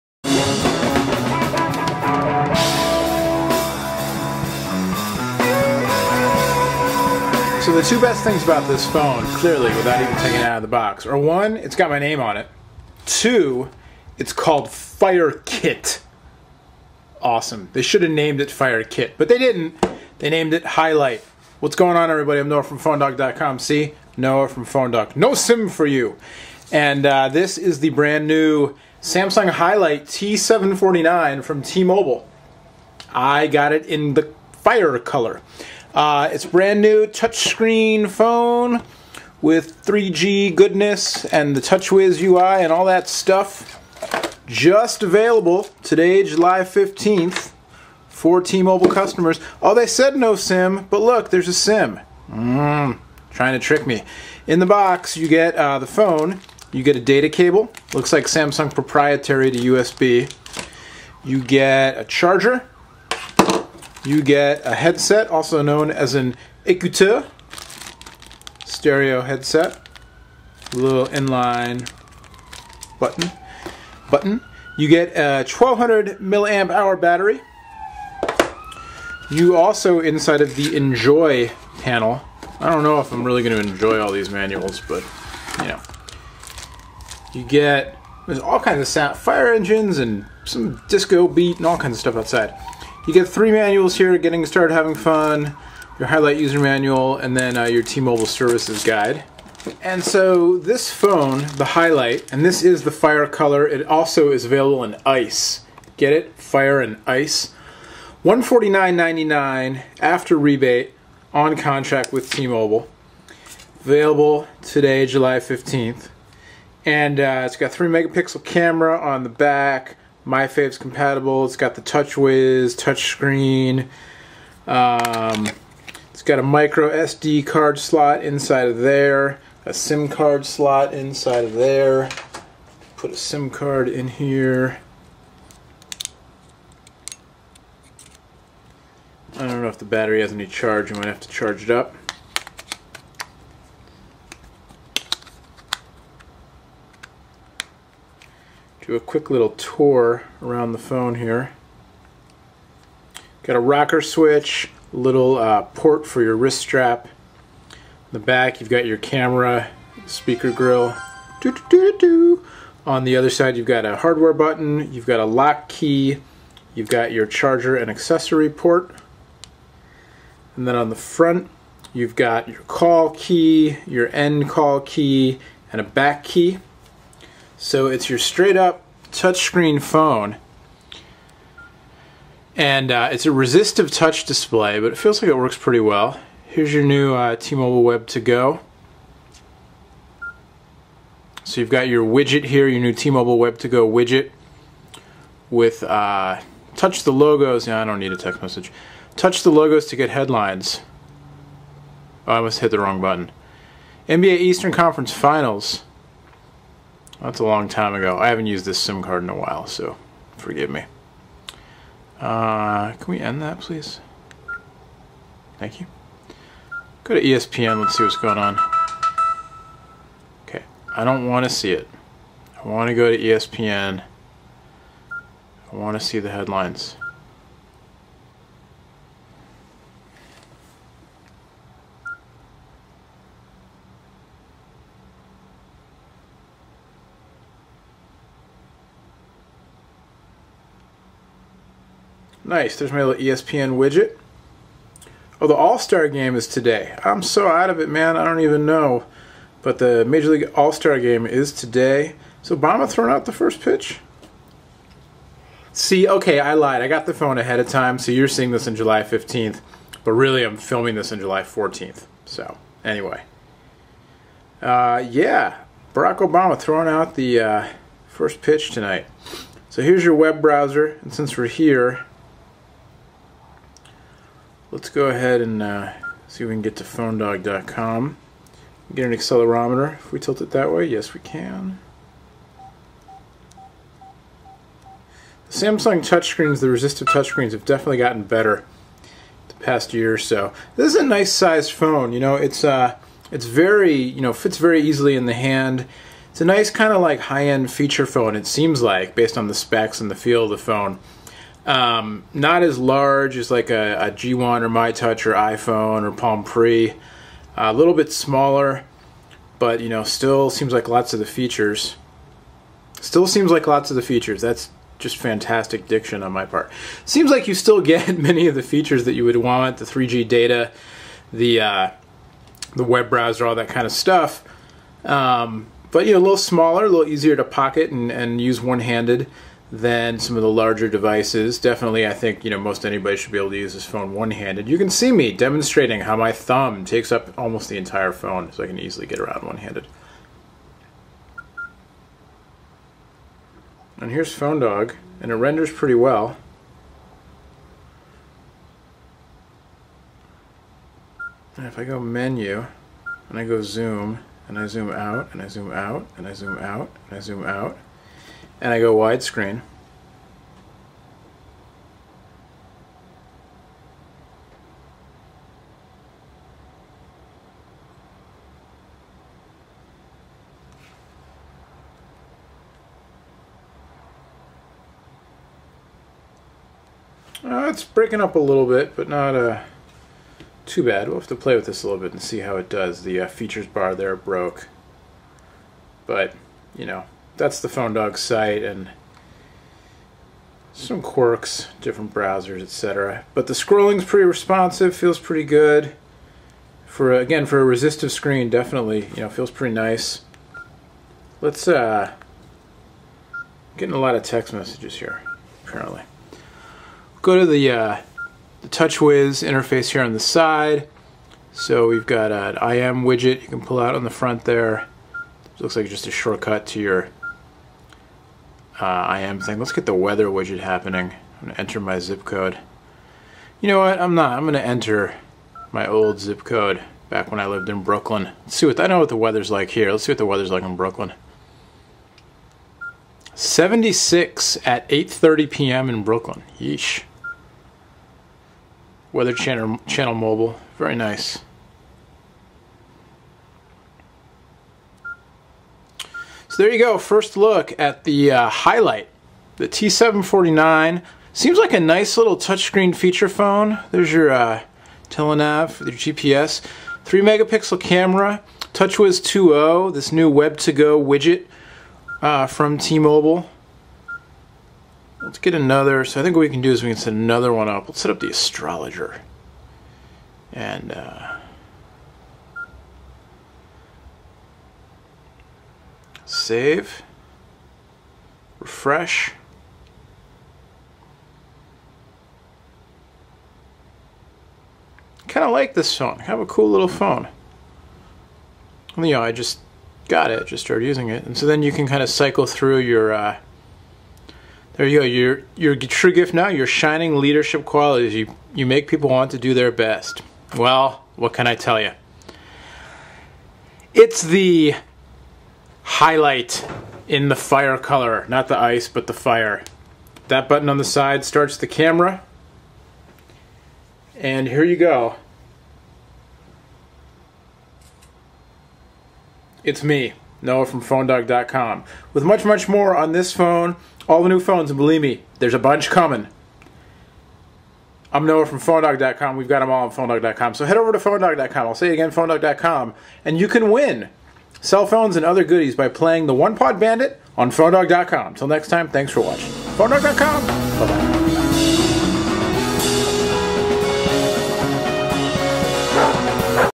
Samsung Highlight T749 (T-Mobile) - Unboxing
T-Mo's new all touch phone comes in Fire and Ice.  Noah got the Fire.  Duh.  Check it out.

Win Free Phones for all the latest cell phone news, reviews, and more.